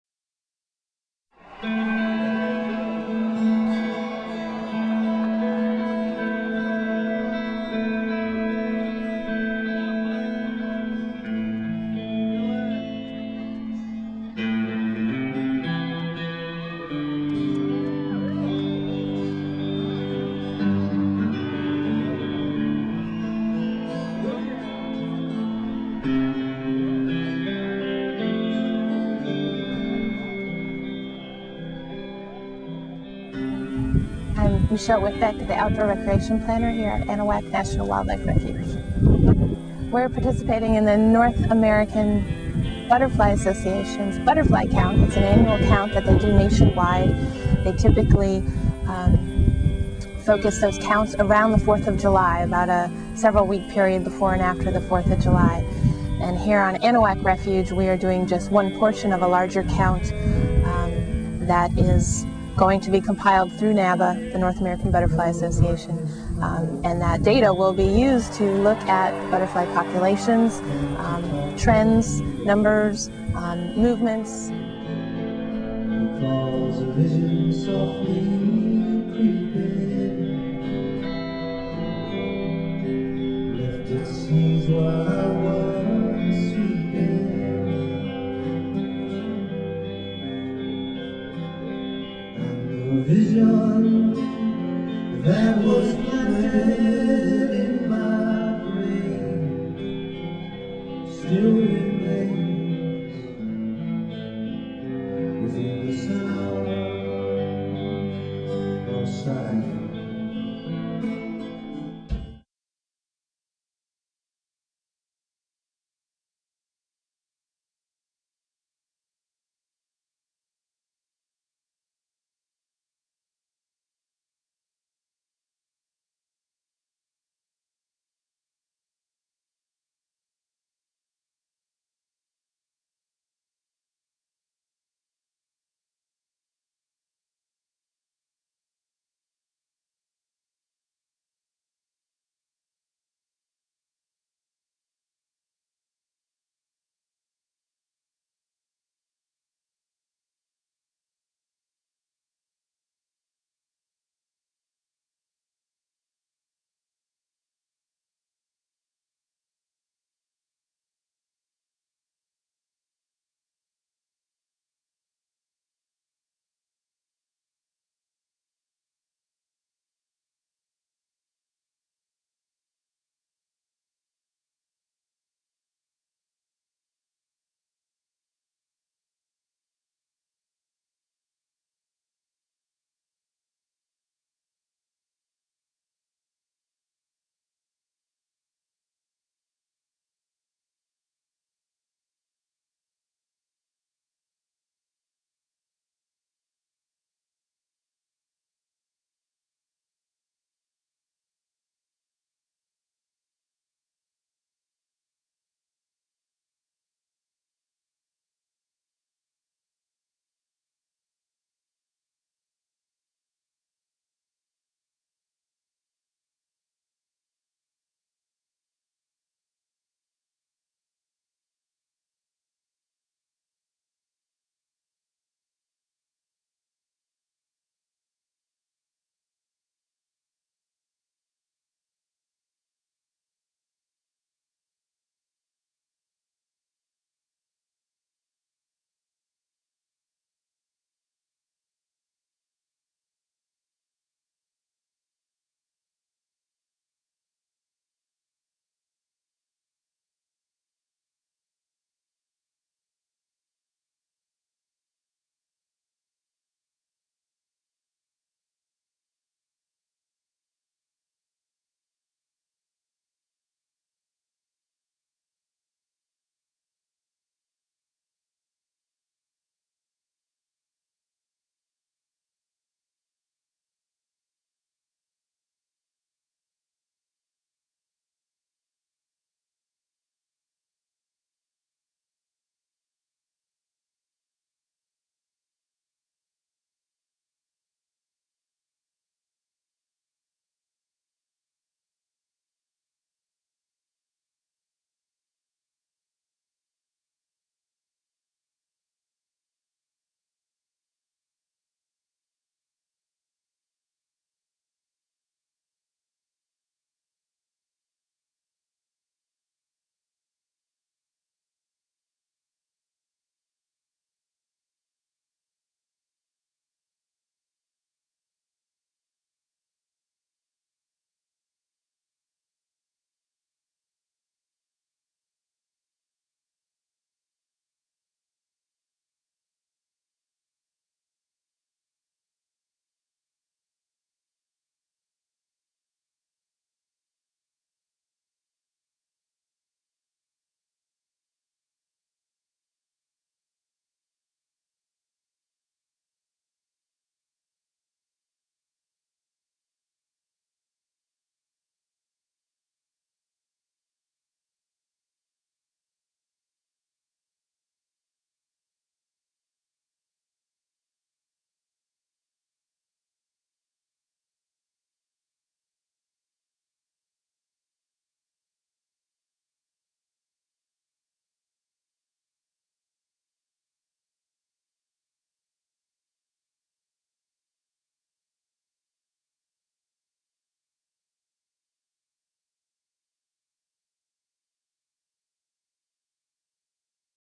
On butterfly's wings
Each year the North American Butterfly Association gathers to count the majestic creatures all across America. Here is a portion of what was found in Southeast Texas